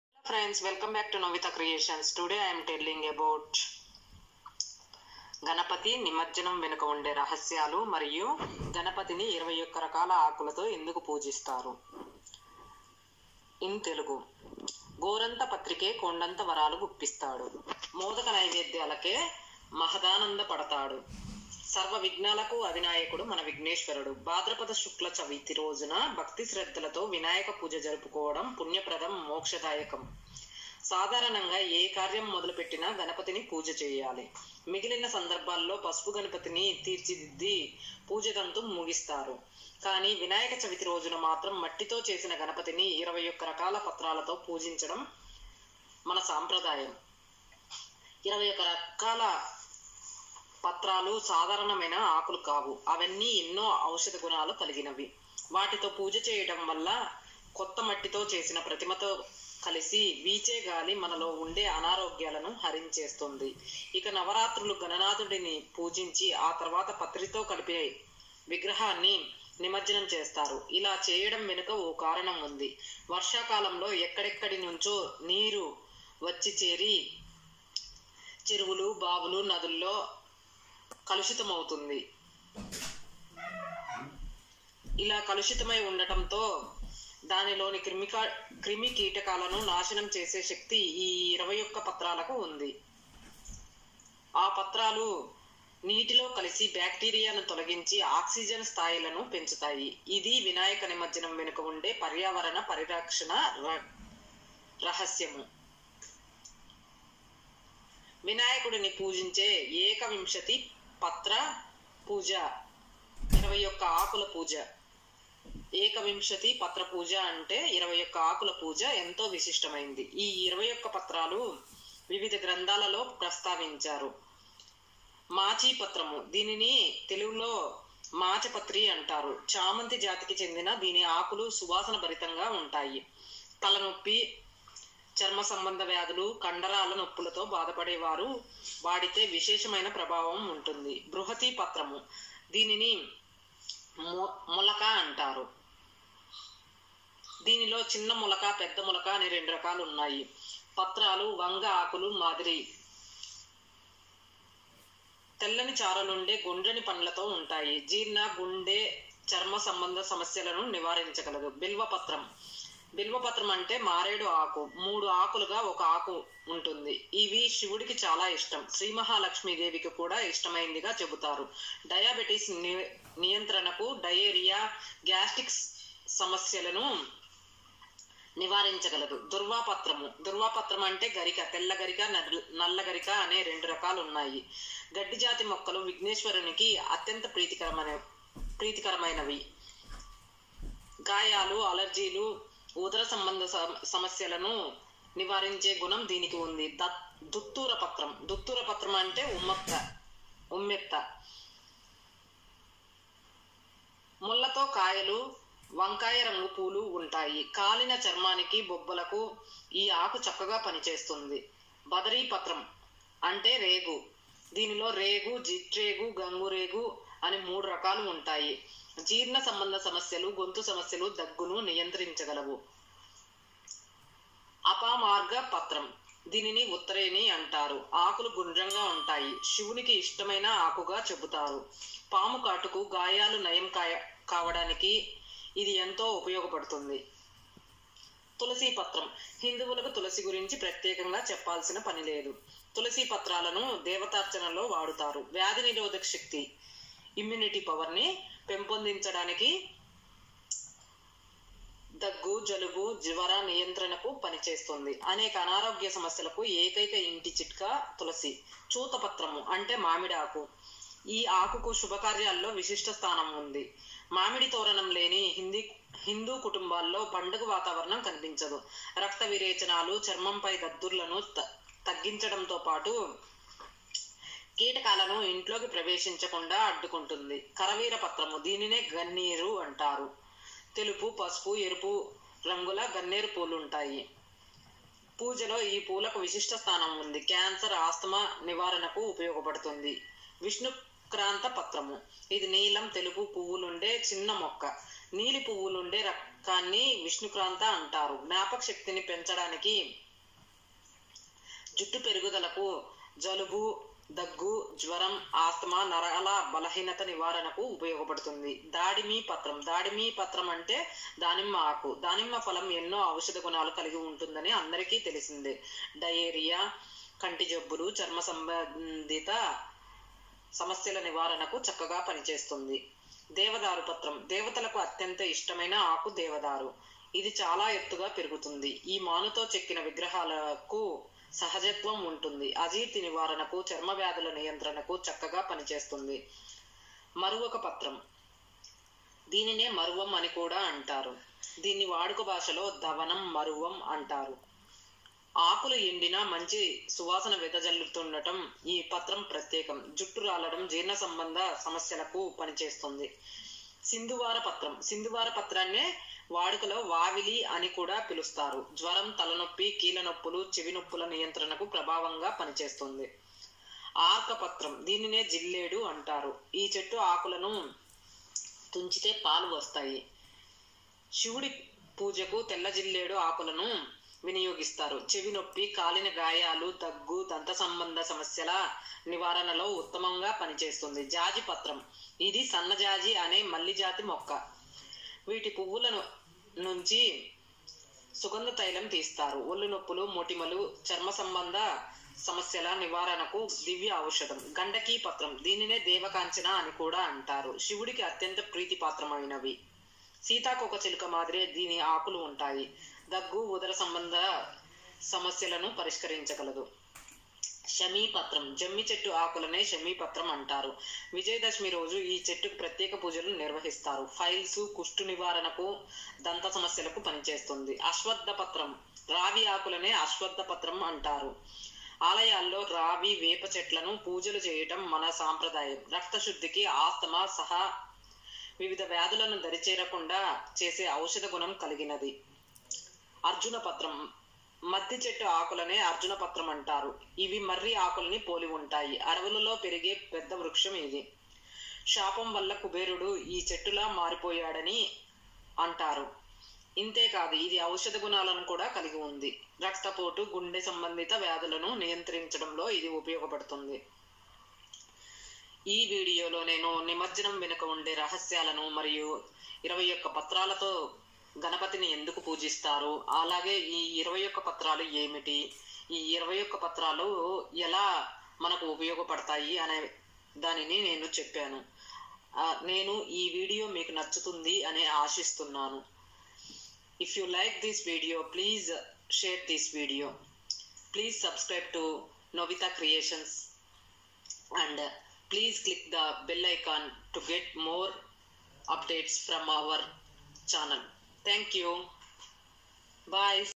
వినాయక నిమజ్జనం వెనక ఉండే రహస్యాలు ఏమిటి# వినాయకుని 21 రకాల పత్రులతో ఎందుకు పూజిస్తారు?
If you like this video, share and comment. Please click bell icon to get future notifications  from us. Thanks👇👇👇